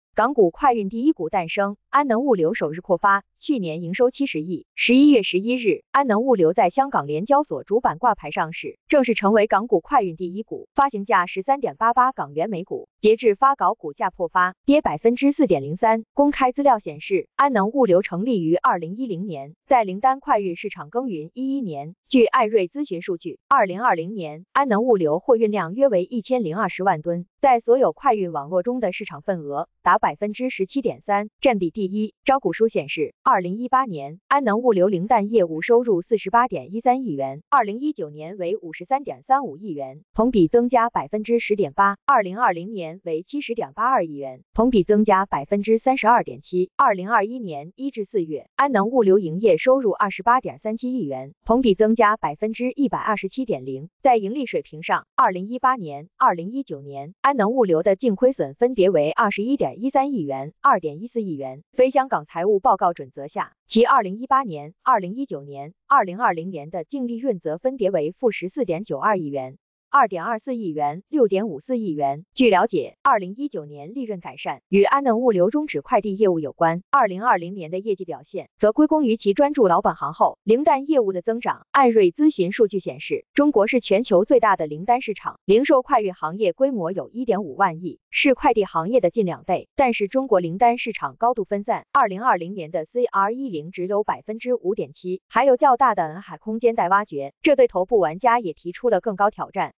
港股快运第一股诞生，安能物流首日破发，去年营收70亿
11月11日，安能物流在香港联交所主板挂牌上市，正式成为“港股快运第一股”，发行价13.88港元/股，截至发稿股价破发，跌4.03%。公开资料显示，安能物流成立于2010年，在零担快运市场耕耘11年。据艾瑞咨询数据，2020年，安能物流货运量约为1020万吨，在所有快运网络中的市场份额达17.3%，占比第一。招股书显示，2018年，安能物流零担业务收入48.13亿元；2019年为53.35亿元，同比增加10.8%；2020年为70.82亿元，同比增加32.7%；2021年1-4月，安能物流营业收入28.37亿元，同比增加127.0%。在盈利水平上，2018年、2019年，安能物流的净亏损分别为21.13亿元、2.14亿元。2020年成功扭亏为盈，净利润2.18亿元。非香港财务报告准则下，其2018年、2019年、2020年的净利润则分别为-14.92亿元、2.24亿元、6.54亿元。据了解，2019年利润改善，与安能物流终止快递业务有关；2020年的业绩表现，则归功于其专注老本行后，零担业务的增长。艾瑞咨询数据显示，中国是全球最大的零担市场，零售快运行业规模有1.5万亿，是快递行业的近2倍。但是中国零担市场高度分散，2020年的CR10只有5.7%，还有较大的蓝海空间待挖掘，这对头部玩家也提出了更高挑战。（文 | AI财经社 刘雪儿 编 | 孙静）